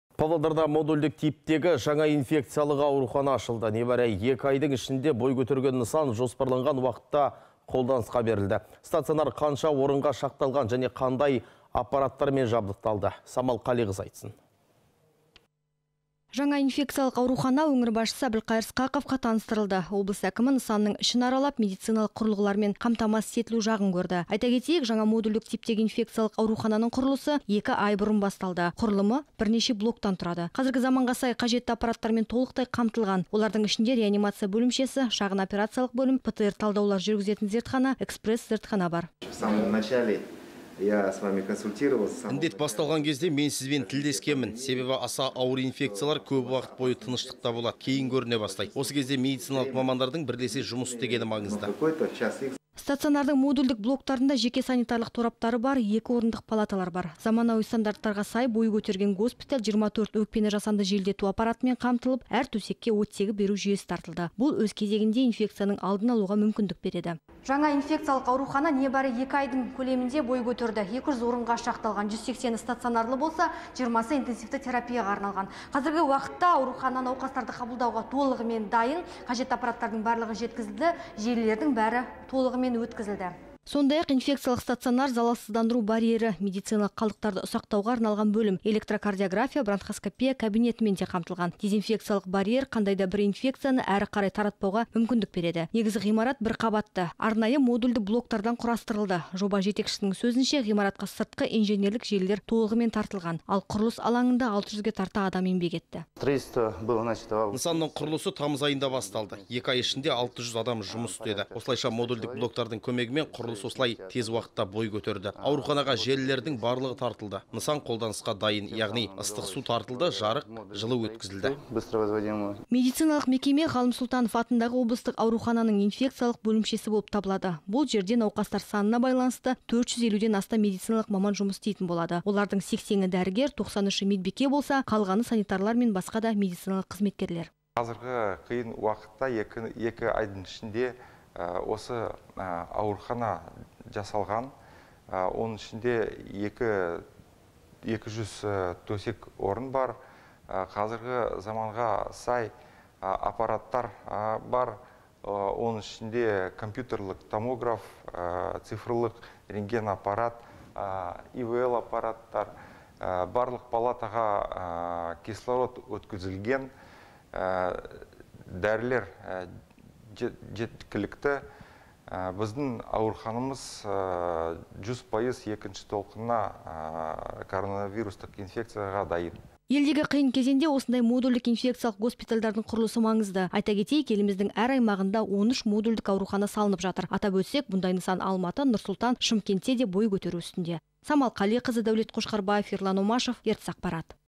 МОДУЛЬДІК ИНФЕКЦИЯЛЫҚ ГОСПИТАЛЬ АШЫЛДЫ
Павлодарда модульдік типтегі жаңа инфекциялық аурухана ашылды. Небəрі екі айдың ішінде бой көтерген нысан жоспарланған уақытта  қолданысқа берілді. Стационар қанша орынға шақталған және қандай аппараттармен жабдықталды?